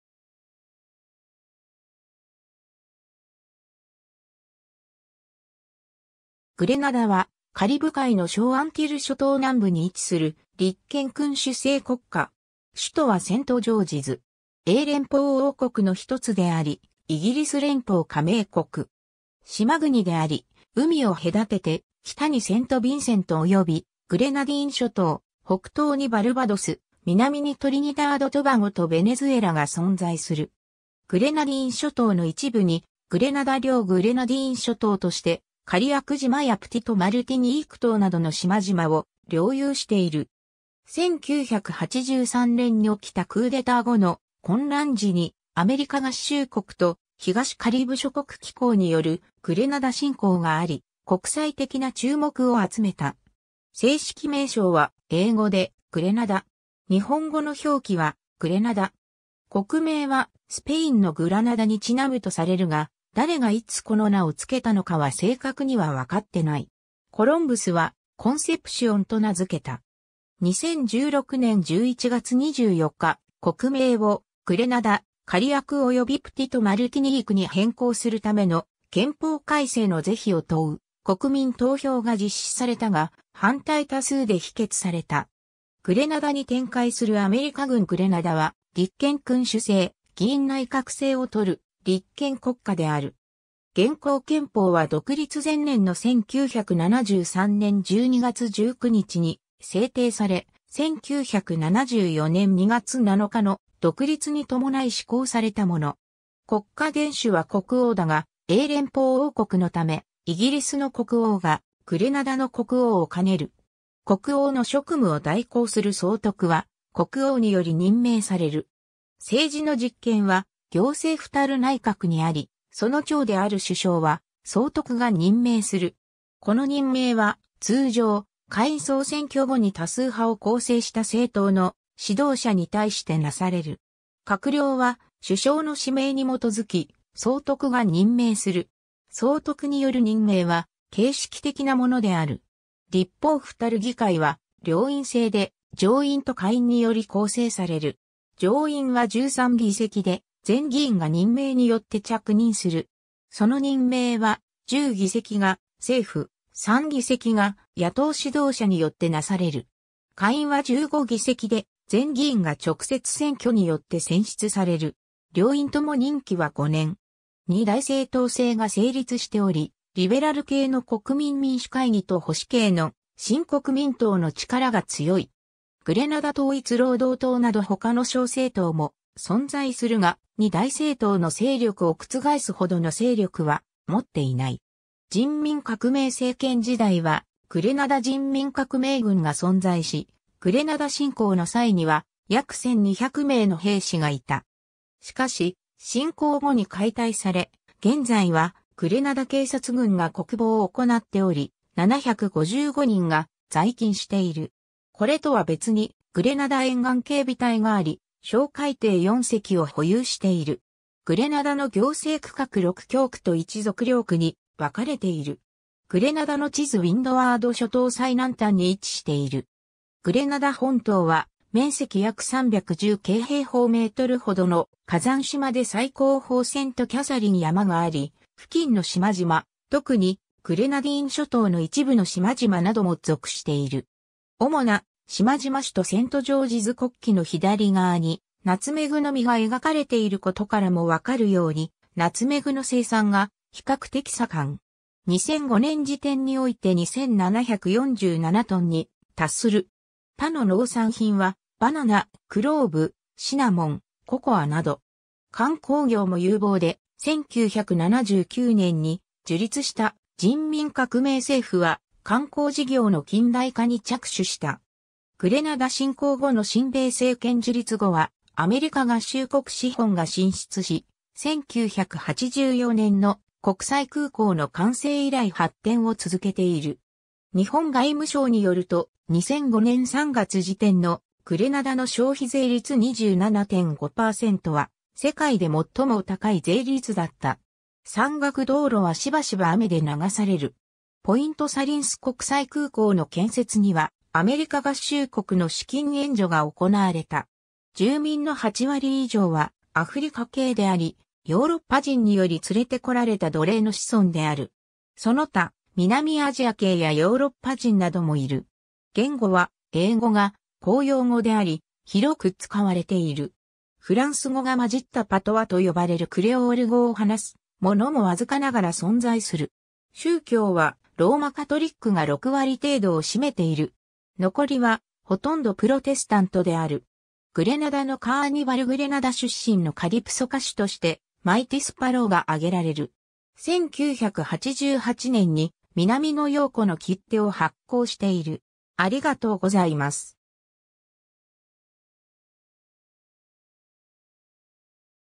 グレナダ
グレナダ（英語: Grenada、グレナダ・クレオール語: Gwenad）は、カリブ海の小アンティル諸島南部に位置する立憲君主制国家。
首都はセントジョージズ。
 英連邦王国のひとつであり、イギリス連邦加盟国。
島国であり、海を隔てて北にセントビンセントおよびグレナディーン諸島、北東にバルバドス、南にトリニダード・トバゴとベネズエラが存在する。
 グレナディーン諸島の一部にグレナダ領グレナディーン諸島として、カリアク島やプティト・マルティニーク島などの島々を領有している。
1983年に起きたクーデター後の混乱時にアメリカ合衆国と東カリブ諸国機構（OECS）によるグレナダ侵攻があり、国際的な注目を集めた。
 正式名称は英語で、Grenada（/ɡɹəˈneɪdə/、グラネイダ 聞く ）。
日本語の表記は、グレナダ。
 国名はスペインのグラナダにちなむとされる（セントビンセント・グレナディーンのグレナディーン諸島も同語源である）が、誰がいつこの名をつけたのかは正確には分かってない。
コロンブスは「コンセプシオン（Concepción）」と名づけた。
 2016年11月24日、国名を「グレナダ、カリアクおよびプティト・マルティニーク（Grenada, Carriacou and Petite Martinique）」に変更するための憲法改正の是非を問う国民投票が実施されたが、反対多数で否決された。
 グレナダに展開するアメリカ軍（グレナダ侵攻） グレナダは立憲君主制（英連邦王国）、議院内閣制をとる立憲国家である。
現行憲法は独立前年の1973年12月19日に制定され、1974年2月7日の独立に伴い施行されたもの。
 国家元首は国王だが、英連邦王国のため、イギリスの国王がグレナダの国王を兼ねる。
国王の職務を代行する総督は、国王により任命される。
政治の実権は行政府たる内閣にあり、その長である首相は総督が任命する。
この任命は通常、下院総選挙後に多数派を構成した政党の指導者に対して成される。
閣僚は首相の指名に基づき、総督が任命する。
総督による任命は形式的なものである。
 立法府たる議会は両院制で、上院と下院により構成される。
上院は13議席で、全議員が任命によって着任する。
その任命は、10議席が政府、3議席が野党指導者によって成される。
下院は15議席で、全議員が直接選挙（小選挙区制）によって選出される。
両院とも任期は5年。
 二大政党制が成立しており、リベラル系の国民民主会議（NDC）と保守系の新国民党（NNP）の力が強い。
グレナダ統一労働党（GULP）など他の小政党も存在するが、二大政党の勢力を覆すほどの勢力は持っていない。
 人民革命政権時代はグレナダ人民革命軍が存在し、グレナダ侵攻の際には約1200名の兵士がいた。
しかし、侵攻後に解体され、現在はグレナダ警察軍が国防を行っており、755人が在勤している。
これとは別にグレナダ沿岸警備隊があり、哨戒艇4隻を保有している。
 グレナダの行政区画 6教区と1属領区に分かれている。
 グレナダの地図 ウィンドワード諸島最南端に位置している。
グレナダ本島は面積約310Km2ほどの火山島で最高峰セント・キャサリン山（840m）があり、付近の島々、特にグレナディーン諸島の一部の島々なども属している。
 主な島々 首都セントジョージズ 国旗の左側にナツメグの実が描かれていることからもわかるように、ナツメグの生産が比較的さかん。
2005年時点において2747トン（世界第6位、シェア3%）に達する（FAO）。
他の農産品はバナナ、クローブ、シナモン、ココアなど。
 観光業も有望で、1979年に樹立した人民革命政府は観光事業の近代化に着手した。
グレナダ侵攻後の親米政権樹立後はアメリカ合衆国資本が進出し、1984年の国際空港の完成以来発展を続けている。
 日本外務省によると、2005年3月時点のグレナダの消費税率27.5%は、世界で最も高い税率だった（2007年以降は6位以下）。
 山岳道路はしばしば雨で流される。
ポイント・サリンス国際空港の建設にはアメリカ合衆国の資金援助が行われた。
 住民の8割以上はアフリカ系であり、ヨーロッパ人により連れて来られた奴隷の子孫である。
その他、南アジア系やヨーロッパ人などもいる。
 言語は英語が公用語であり、広く使われている。
フランス語が混じったパトワと呼ばれるクレオール語を話すものもわずかながら存在する...